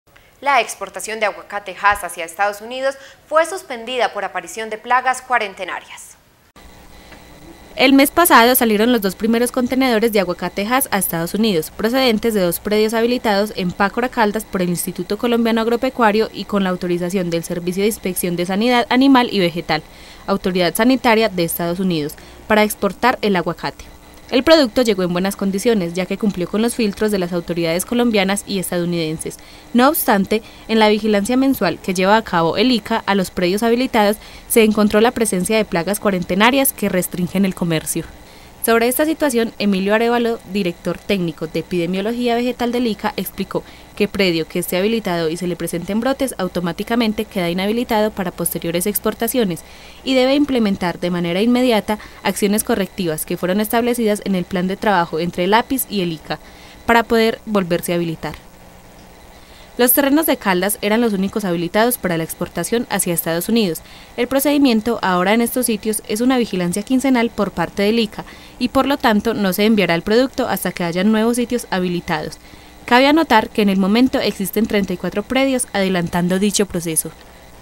PLAGAS FRENARON LA EXPORTACIÓN DEL AGUACATE HASS A EEUU
La exportación de aguacate hass hacia los Estados Unidos fue suspendida por la aparición de plagas cuarentenarias.